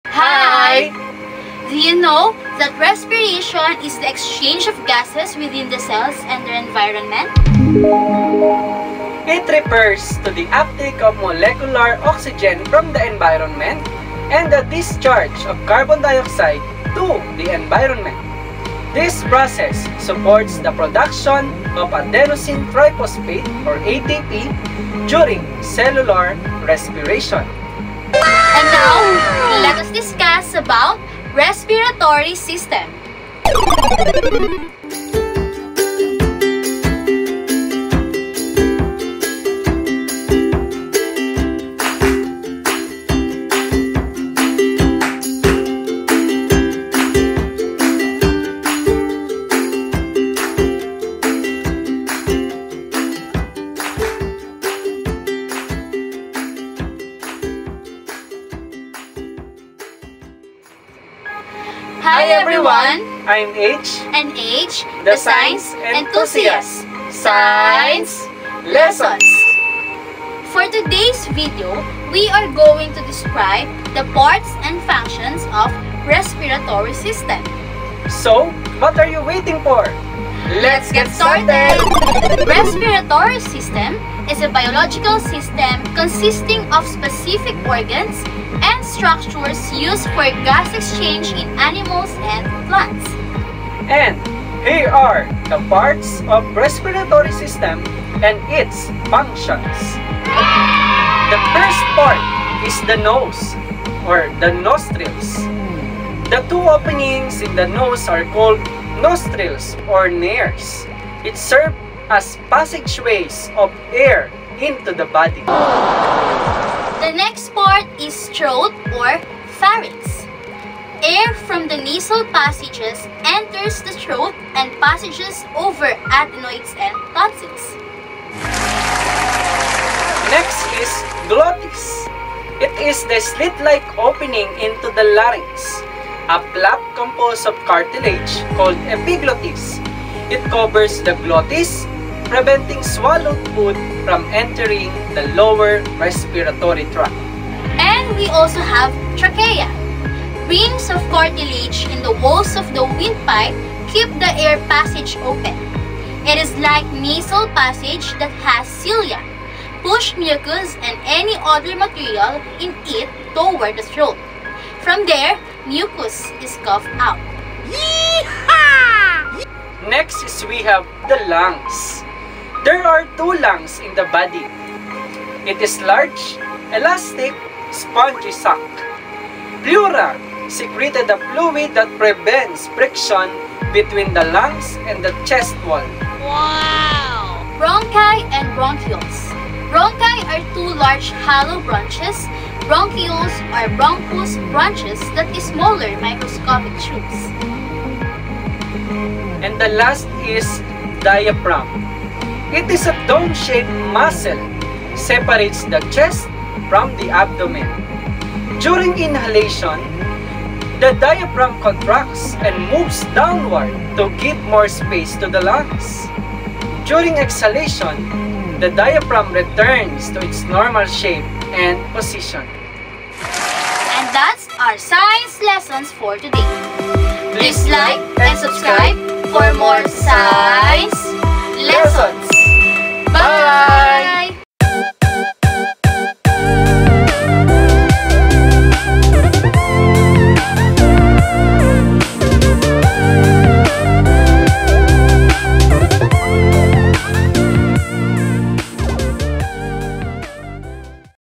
H&H: The Science Enthusiasts – RESPIRATORY SYSTEM || Biology Lesson
RESPIRATORY SYSTEM 👃👃👨‍🔬👩‍🔬🌳🧬🦠🩺

🩺RESPIRATION - is the exchange of gases within the cells and their environment. It refers to the uptake of molecular oxygen from the environment and the discharge of carbon dioxide to the environment. This process supports the production of adenosine triphosphate (ATP) during cellular respiration.

🩺RESPIRATORY SYSTEM - is a biological system consisting of specific organs and structures used for gas exchange in animals and plants.

THE PARTS OF RESPIRATORY SYSTEM AND ITS FUNCTIONS 🧬🦠

👃NOSE (Nostrils) - The two openings in the nose are called Nostrils (or nares). It serve as passageways of air into the body.

👃THROAT or PHARYNX - Air from the nasal passages enters the throat and passages over adenoids and tonsils.

👃GLOTTIS - is the slit-like opening into the larynx. A flap composed of cartilage called epiglottis covers the glottis, preventing swallowed food from entering the lower respiratory track.

👃TRACHEA - Rings of cartilage in the walls of the windpipe keep the air passage open. Is like nasal passage that has cilia. (push mucus and any other material in it toward the throat). From there, mucus is coughed out.

👃LUNGS - There are two lungs in the body. It is large, elastic, spongy sac. Pleura secreted a fluid that prevents friction between the lungs and the chest wall.

👃BRONCHI and BRONCHIOLES - Bronchi are two large hollow branches.
Bronchioles are bronchus branches that is smaller microscopic tubes.

👃DIAPHRAGM - dome-shaped muscle separates the chest from the abdomen. During inhalation, the diaphragm contracts and moves downward to give more space to the lungs. During exhalation, the diaphragm returns to its normal shape and position.